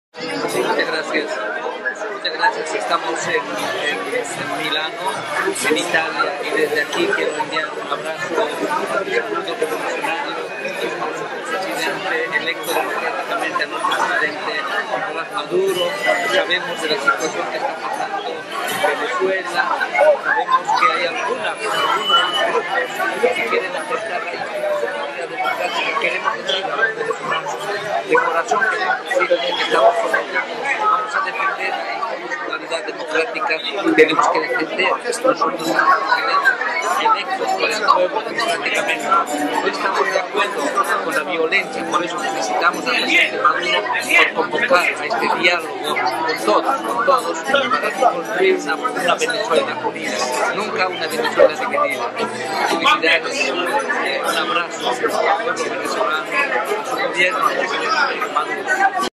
Canciller David Choquehuanca envía mensaje de Solidaridad al Pueblo Revolucionario de Venezuela y a
Transcripción del mensaje: Muchas gracias, estamos en Milano, en Italia, y desde aquí quiero enviarle un abrazo y un saludo revolucionario a vuestro Presidente electo democráticamente, vuestro Presidente Nicolás Maduro. Sabemos la situación que está pasando Venezuela, sabemos que hay algunas y algunos grupos que quieren afectar la institucionalidad democratica; queremos decirle a los venezolanos, de corazón queremos decirles que estamos con ellos, vamos a defender la institucionalidad democrática; tenemos que defender nosotros a los presidentes electos por el pueblo democráticamente. No estamos de acuerdo con la violencia, por eso felicitamos al Presidente Maduro por convocar a este dialogo con todos, con todos para construir una Venezuela unida, nunca una Venezuela dividida. Felicidades, un abrazo al pueblo venezolano, a su gobierno y al Presidente Maduro